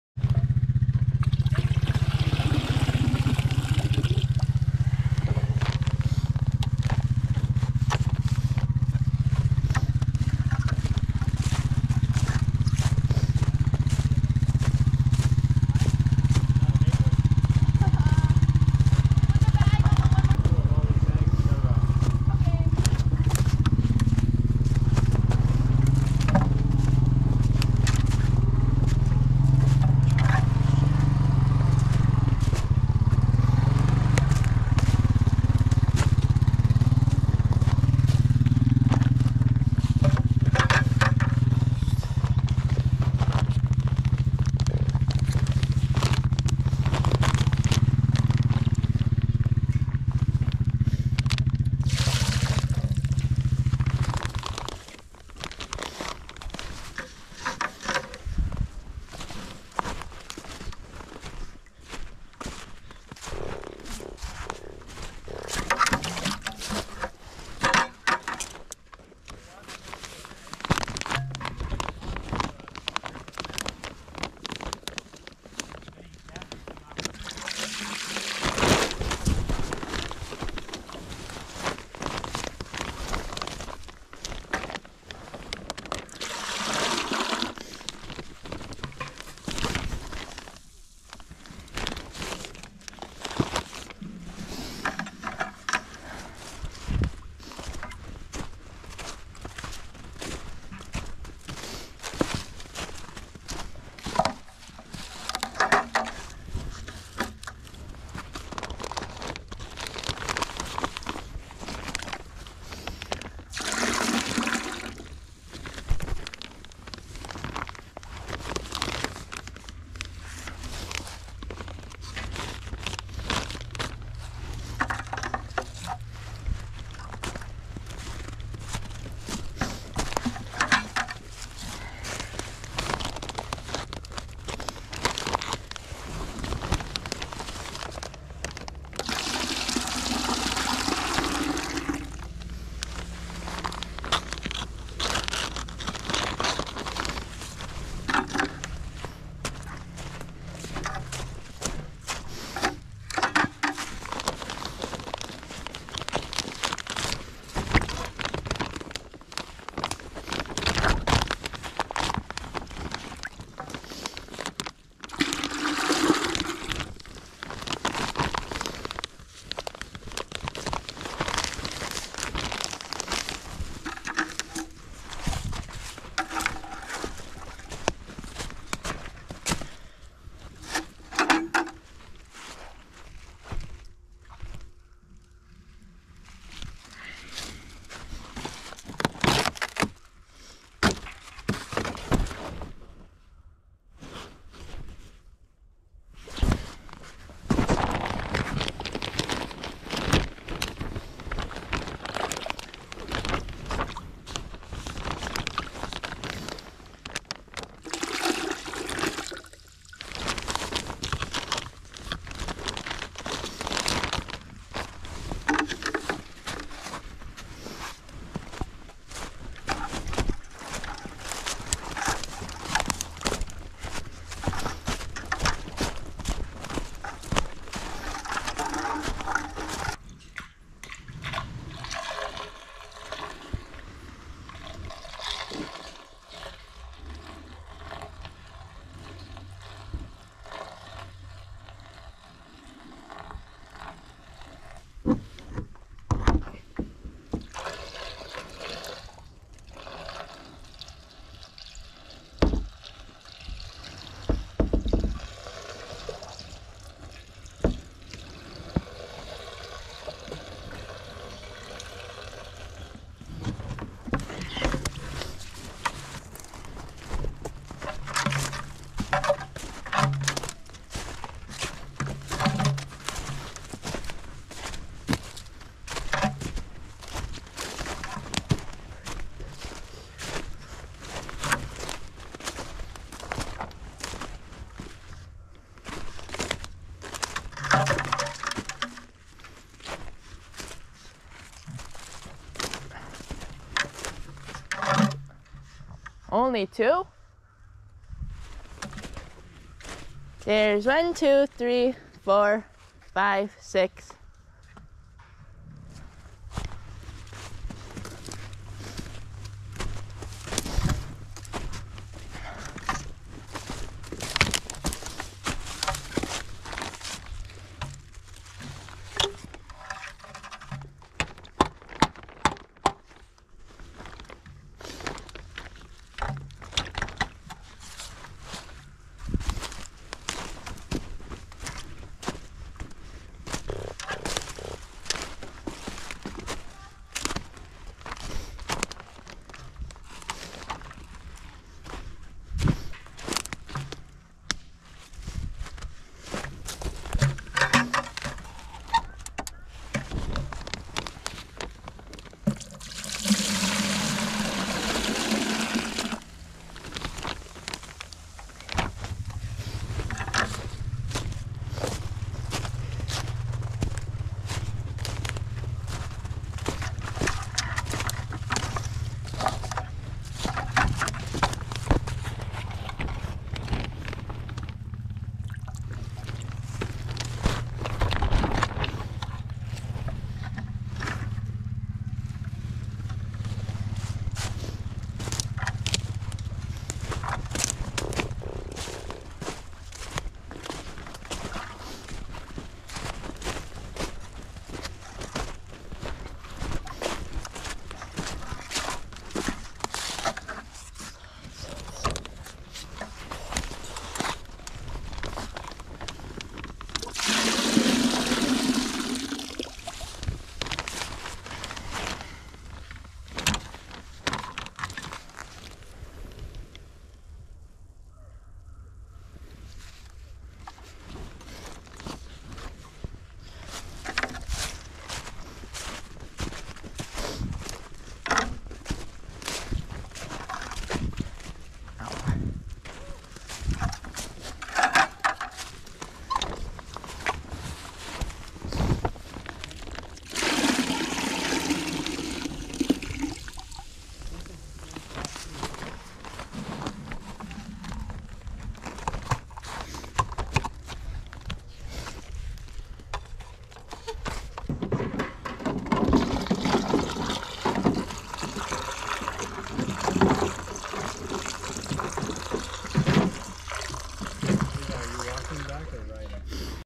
collecting maple sap
me collecting and pouring maple sap to make maple syrup 🍁